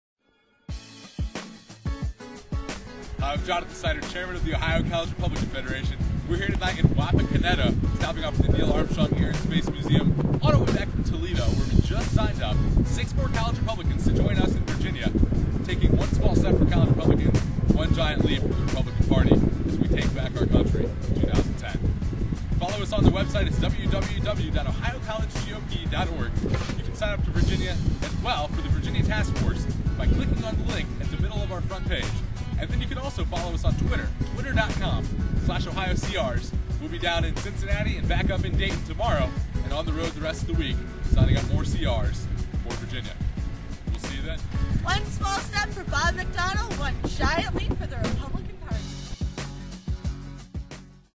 OhioCRs Campus Tour 2009 Trying to recruit Neil Armstrong for the VA Task Force!
Driving back from Toledo, we decided to grab Neil Armstrong for our trip to Virginia, but unfortunately all that was at his museum were the Apollo capsules. So we did the next best thing, shoot a video. and click on the CR link to join up! We've traveled over 4500 miles across the state now, helping to recruit over 1500 new College Republicans in Ohio and signing up 60 CRs to deploy to Virginia as part of the CR Task Force. or on Twitter.com/OhioCRs, where you can also track the tour by using the hashtag OHCampusTour.